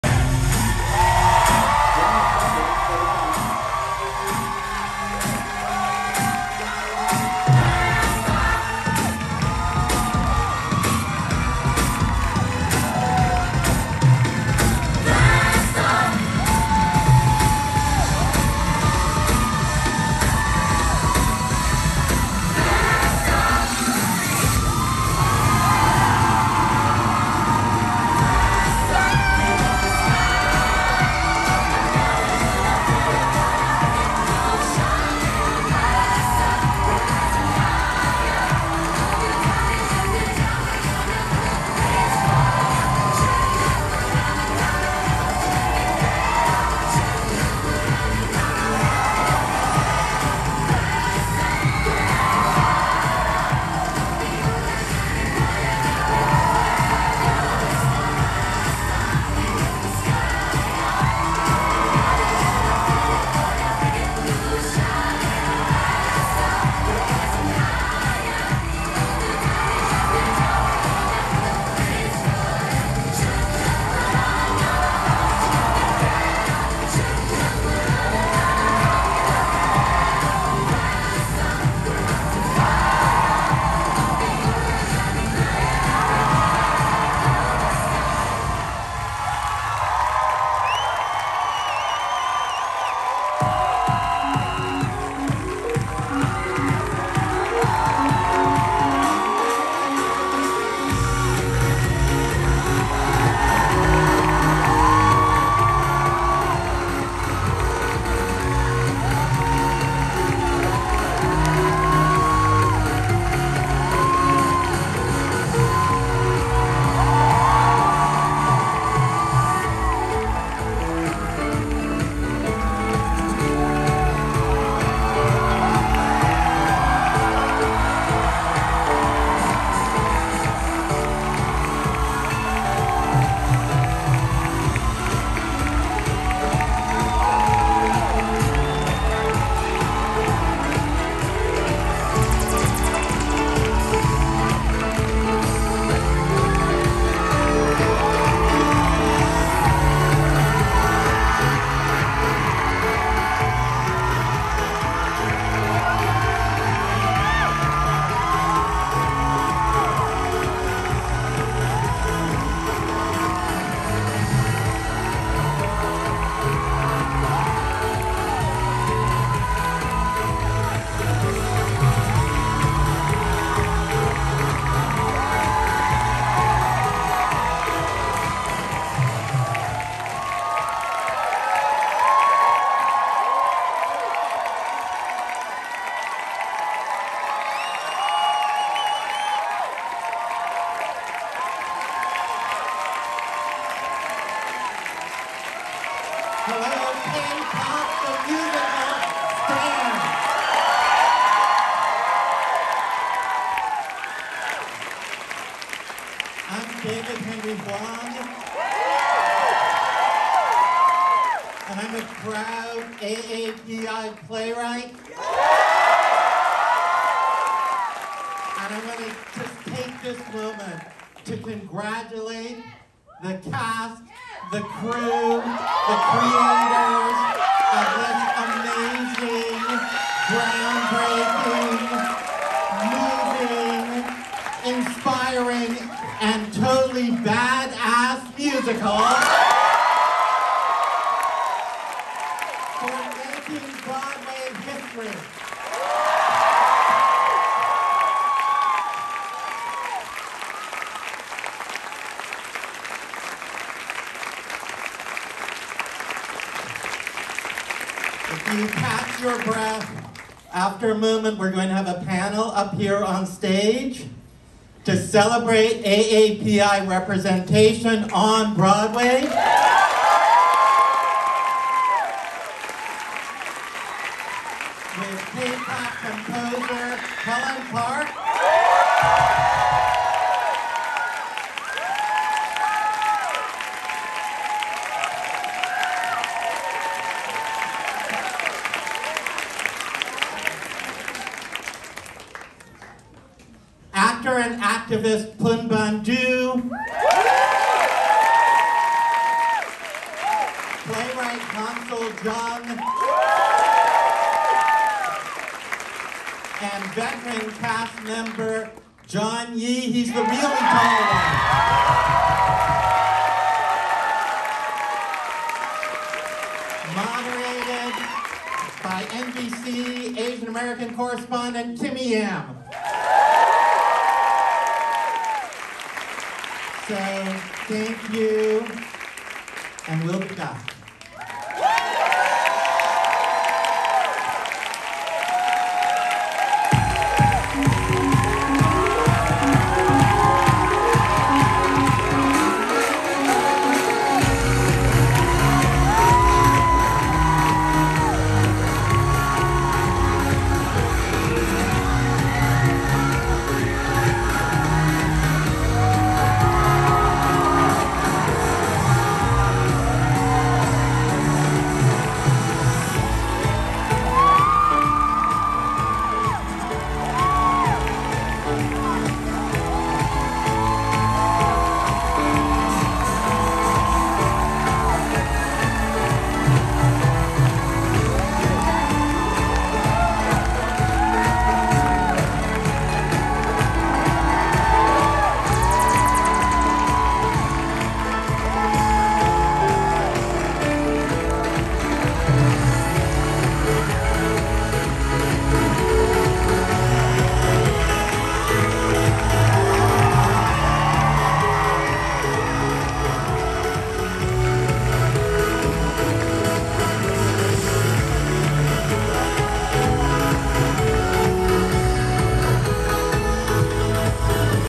Kpop on Broadway Musical - Final Performance Bows - Circle in the Square Theatre - 12/11/22
Kpop Musical on Broadway - final performance bows and speech.

So sad to see this show close so early! It was a lot of fun and I’m happy I saw first previews and the final show 🥹🥲

Show stars Luna from f(x) as MwE.

Circle in the Square Theatre - New York NY